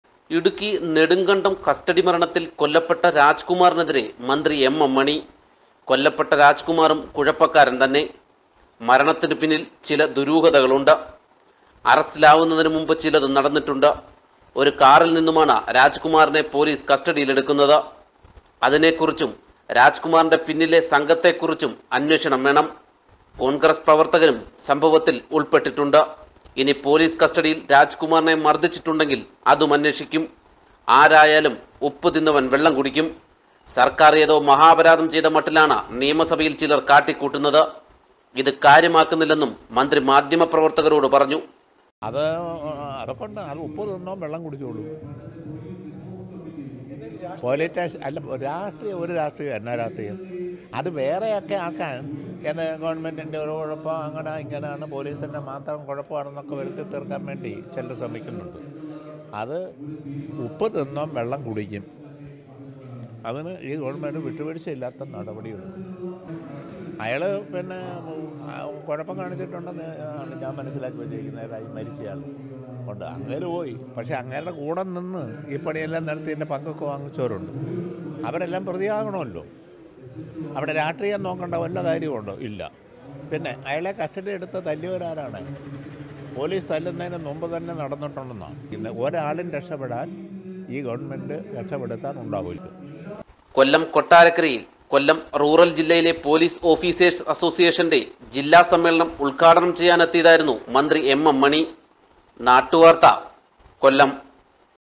mm mani says that rajkumar is a trouble
കസ്റ്റഡി മരണം : മരിച്ച രാജ്കുമാര്‍ കുഴപ്പക്കാരന്‍ തന്നെ .മന്ത്രി മണി